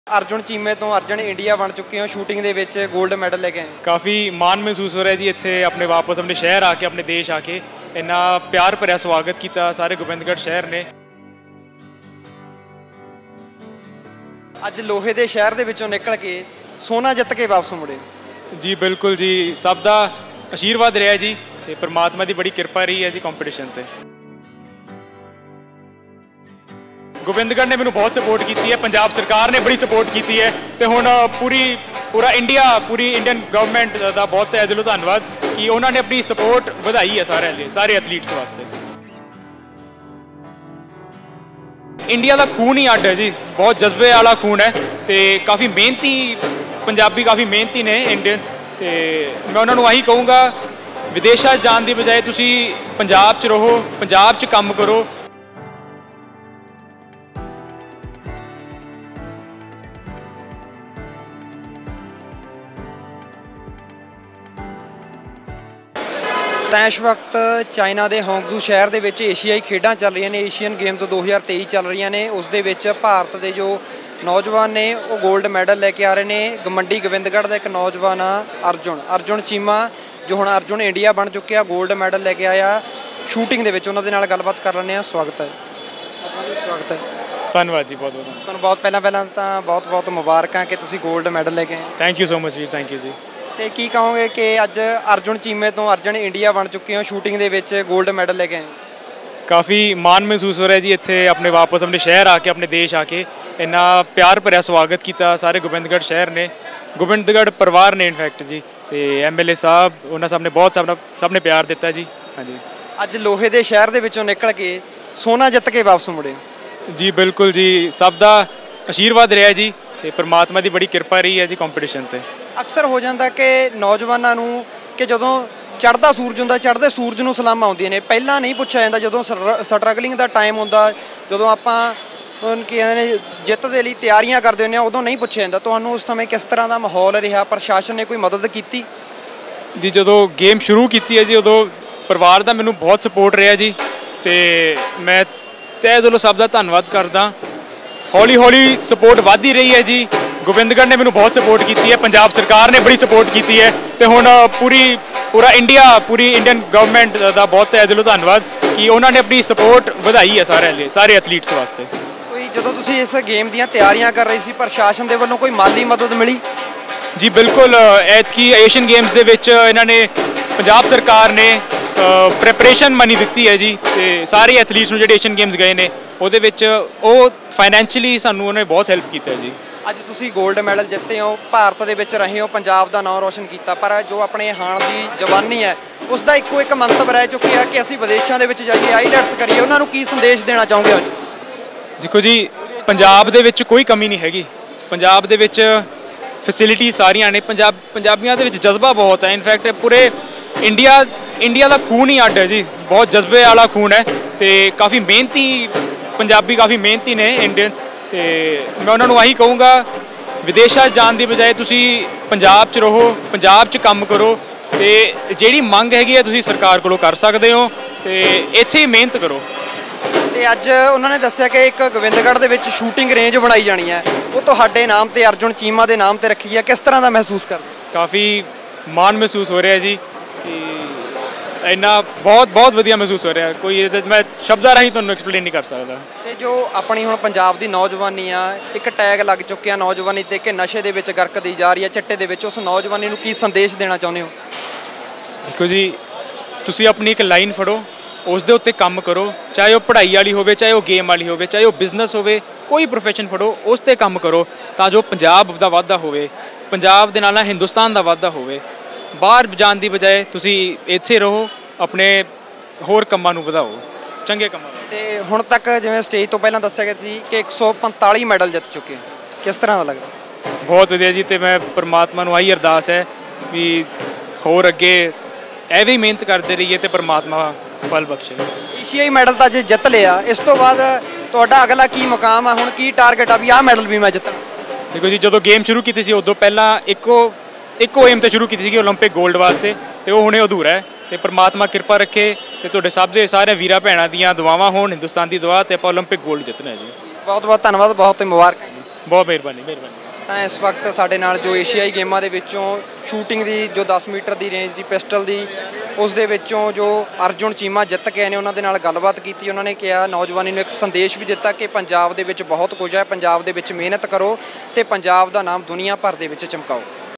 ਏਸ਼ੀਆਈ ਖੇਡਾਂ 'ਚ ਸੋਨ ਤਗਮਾ ਜੇਤੂ ਅਰਜੁਨ ਚੀਮਾ ਨਾਲ ਖ਼ਾਸ ਗੱਲਬਾਤ | Punjab News TV
ਏਸ਼ੀਆਈ ਖੇਡਾਂ 'ਚ ਸੋਨ ਤਗਮਾ ਜੇਤੂ ਅਰਜੁਨ ਚੀਮਾ ਨਾਲ ਖ਼ਾਸ ਗੱਲਬਾਤ